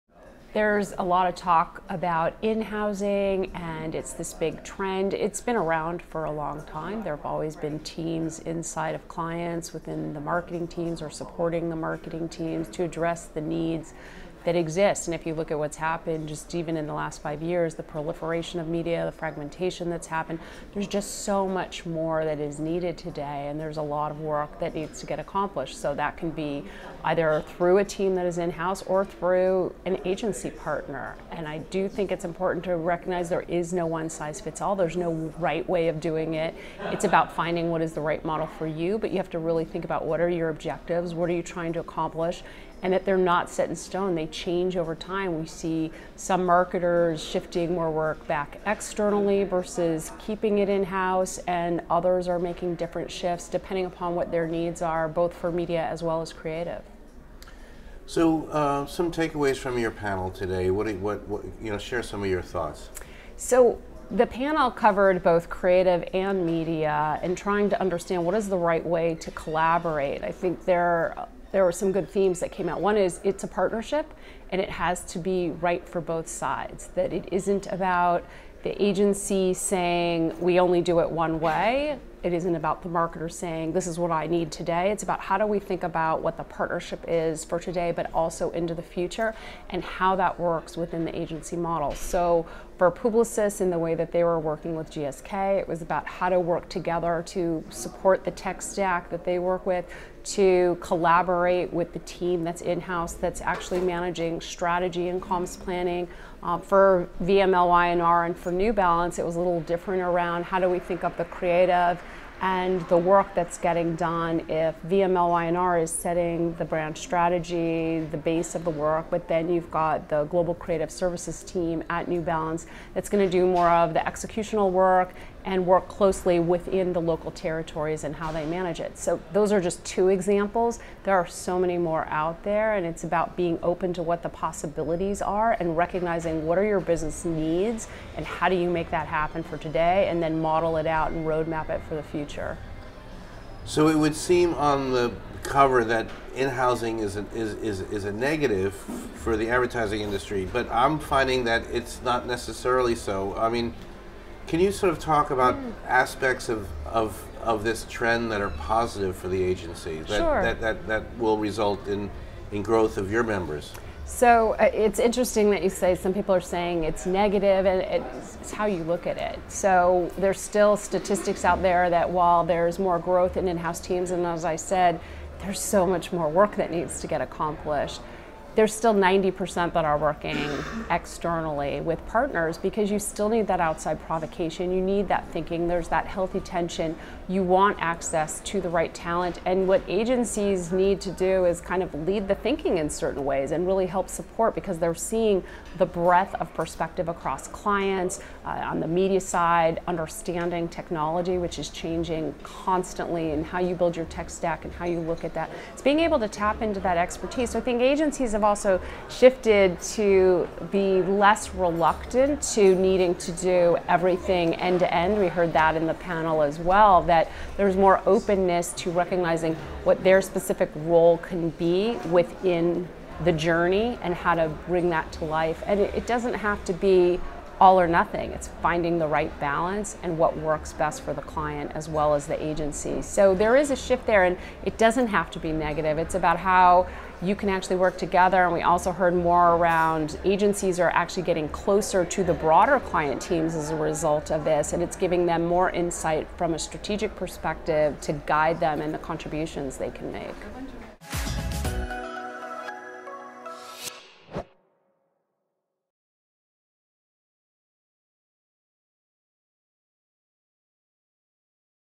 Marketers Should Seek 'The Right Model' When Deciding What To Do In-House: 4A's Kaplowitz
ORLANDO—Before digital media and all of its complexity came along, advertising agencies were basically ensured of an end-to-end relationship with their clients. Now there’s so much more work to be done, it’s not surprising that more marketers are taking certain functions in-house, according to 4A’s President & CEO Marla Kaplowitz.

“It’s been around for a long time. There have always been teams inside of clients within the marketing teams or supporting the marketing teams to address the needs that exist,” Kaplowitz says in this Beet.TV interview at the recent Association of National Advertisers’ In-House Agency Conference.

At the event, Kaplowitz moderated a panel discussion titled Agencies and the Velocity of Change. It brought together executives from VMLY&R and GlaxoSmithKline, plus Publicis Media and New Balance. Turns out “balance” is a key word in the modern-day world of marketers and how they mix internal and external resources.

“There’s just so much more that is needed today and there’s a lot of work that needs to get accomplished,” says Kaplowitz. “I do think it’s important to recognize that here is no one size fits all. There’s no right way of doing it.”

It’s more about “finding what is the right model for you, but you have to really think about what are your objectives, what are you trying to accomplish. And that they’re not set in stone. They change over time.”

One theme from Kalowitz’s panel is that client-agency partnerships have to be right for both parties. For VMLY&R and GSK, a key element was “how to work together to support the tech stack that they work with, to collaborate with the team that’s in-house that’s actually managing strategy and comms planning.” In the case of Publicis and New Balance, while the agency sets the “base” brand strategy, the global creative services team at New Balance does “more of the executional work and work closely within the local territories and how they manage it.”

Asked about negative and positive implications for agencies, Kaplowitz says that’s largely in the eye of the beholder. The recent growth of in-house teams is undeniable, but 90% of marketers still require outside resources.

“Agencies have also shifted to being less reluctant to needing to do everything end to end. There’s more openness to recognizing what their specific role can be within the journey and how to bring that to life. It doesn’t have to be all or nothing. It doesn’t have to be negative.”

This video is part of Beet.TV’s coverage of the ANA In-House Agency Conference. For more videos please visit this page.